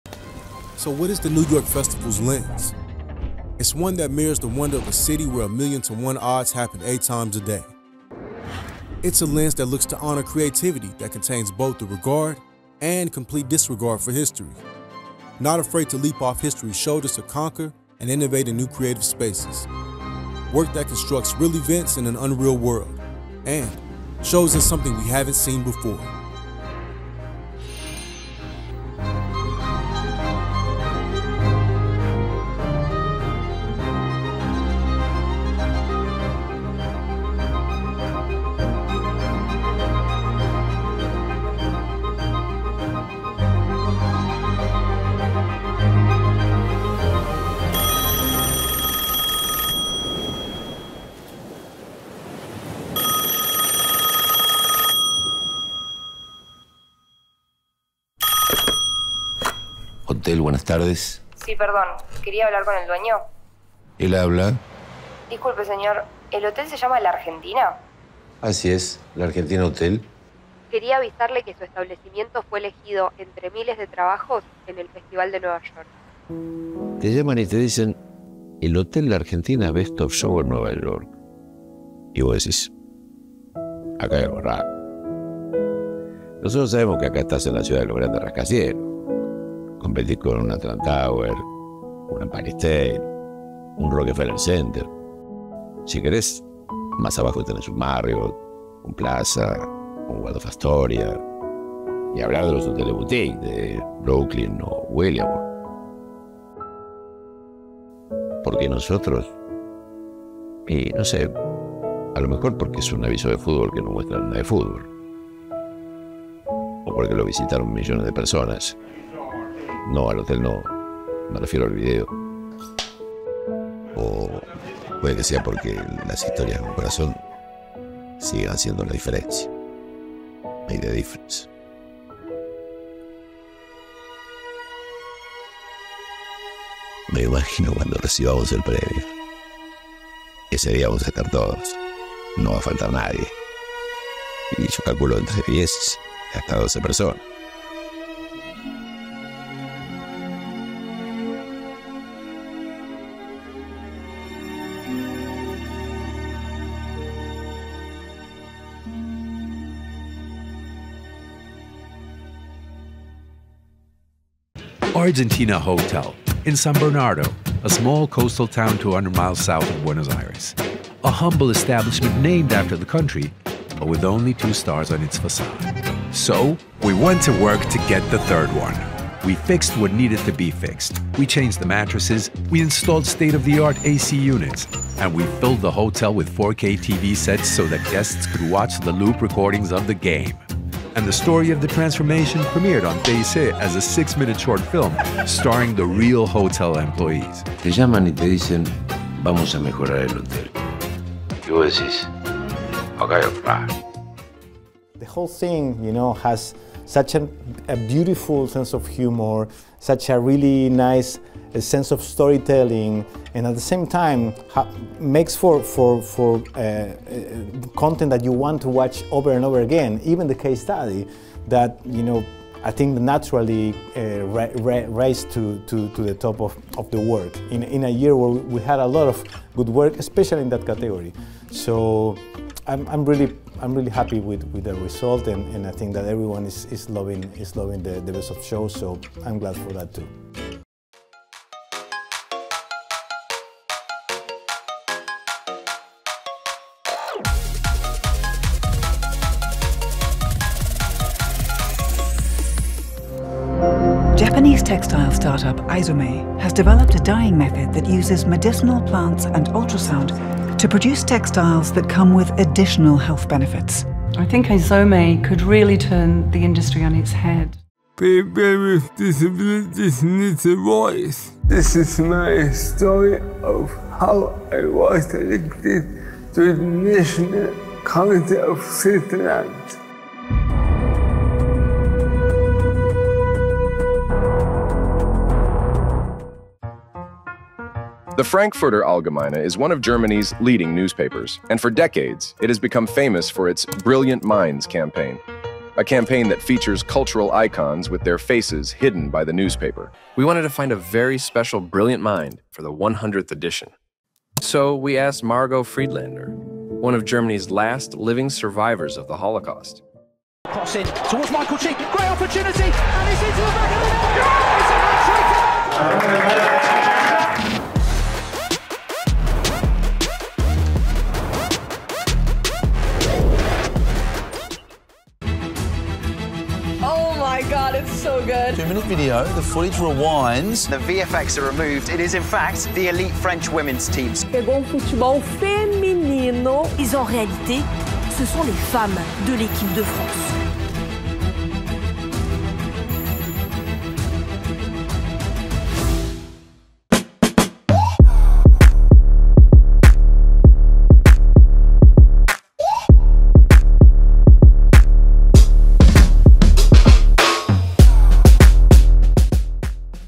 NYFA 2024 Winners Reel
NYF’s Executive Juries, led by Jury President Javier Campopiano, Worldwide Chief Creative Officer of McCann Worldgroup and McCann, have cast their votes and after thoughtful consideration and thorough review…the results are in!

We are proud to announce the 2024 Award-Winners!

Top-scoring campaigns from 60 countries around the globe created by prominent agencies impressed NYF’s powerhouse juries. Trailblazing work that engaged consumers, boosted brand affinity, and delivered ROI earned accolades and NYF Towers.

New York Festivals salutes 2024’s winners. Here’s a behind-the-scenes look at the Grand Prize winners, our Executive Jury judging sessions, and a special message from our Best of Show winner!

Best of Show:
Hotel La Argentina, TyC Sports, Mercado McCann

Grand Awards:
The Everyday Tactician, XBOX, McCann London, McCann Health London, an IPG Health Company

Giving a Voice to the Unheard, Islam Alijaj, Farner | TEAM FARNER

Aizome Wastecare™ Industrial waste — certified as skincare., AIZOME, SERVICEPLAN GERMANY

No Smiles, McDonald's Japan, TBWA HAKUHODO

WoMen's Football, Orange, Marcel

The 100th Edition, Frankfurter Allgemeine Zeitung, Scholz & Friends

Mercado McCann Best of Show Acceptance Video Credits:
Nicolás Massimino, Chief Creative Officer
Manuel Scher, ECD
Edwin Bowen, ECD
Cristian Tarzi, ECD
Agustín Coste, Managing Director
Leila Concistre, Group Account Director
Carolina Scordo, Account Supervisor
Mariano Malvasio, CMO
Germán Vago, Marketing Manager
Daniela Gaspar, Communications & Media Coordinator
Agustín Borgognoni, Head of Production
Carolina García Dias, Producer
Carmen Bistrian, MW Global Product Excellence Director
Emiliano Gonzalez De Pietri, CCO McCann Spain
Lourdes Alderete, Latam MW Creative Excellence Manager
 Diego Peskins, Executive Producer
Martín de Escalada, Director
Mercedes Eliçabe, Post Production Coordinator
Andrés Boero, Editing
Malena Gala, Editing Assistant
Luciano Grispino, Sound
Juan Hirak, VFX
Anahí Piccinin, Color